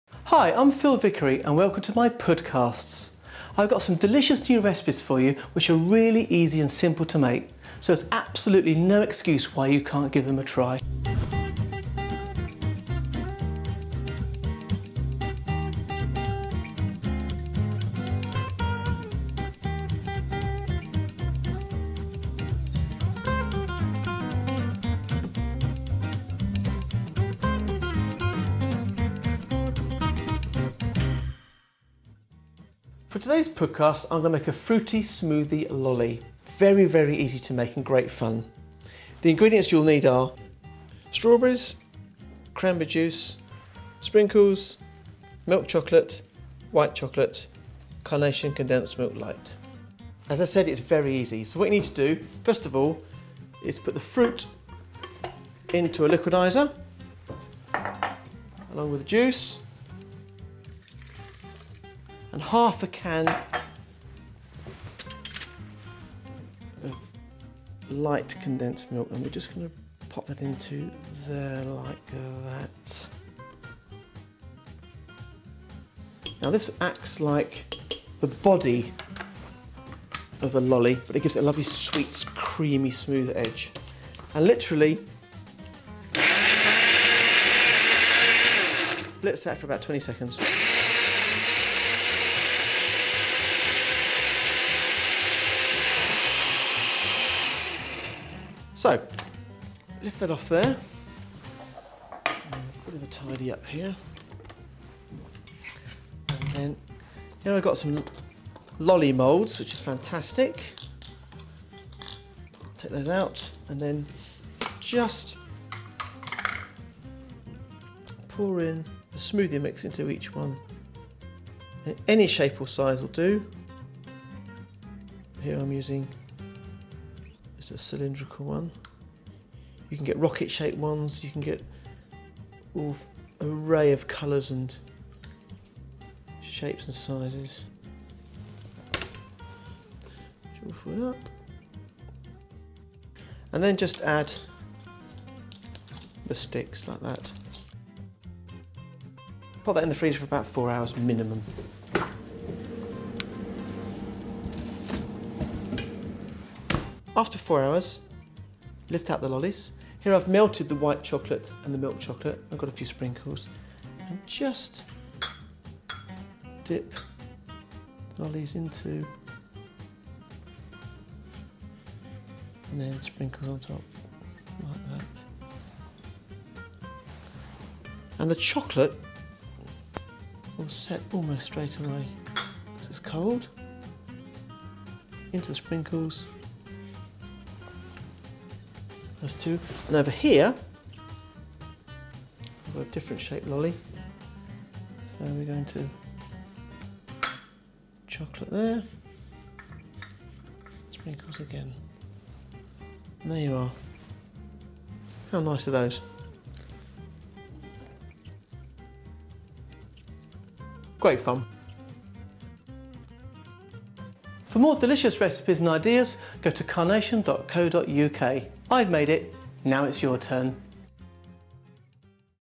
Phil Vickery Makes Refreshing Fruity Smoothie Lollies
This month, celebrity chef and Carnation ambassador, Phil Vickery creates a fun twist on the classic lolly -- perfect for kids and adults alike.  Distributed by Tubemogul.